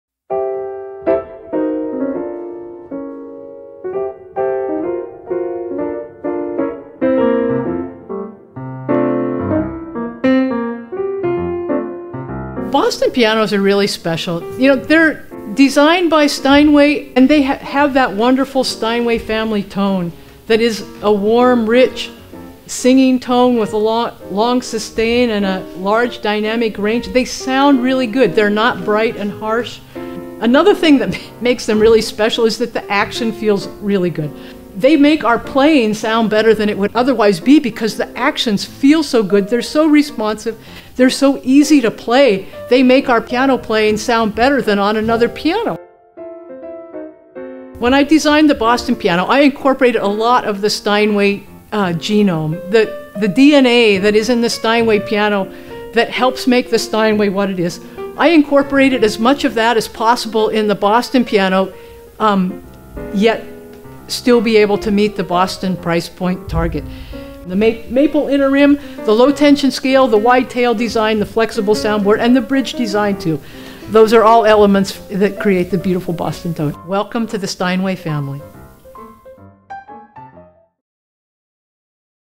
Senior Mechanical Engineer Susan Kenagy on the Characteristics of Boston Pianos
"When I designed the Boston piano I incorporated a lot of the Steinway genome." - Senior Mechanical Engineer Susan Kenagy about the characteristics of our Boston grands.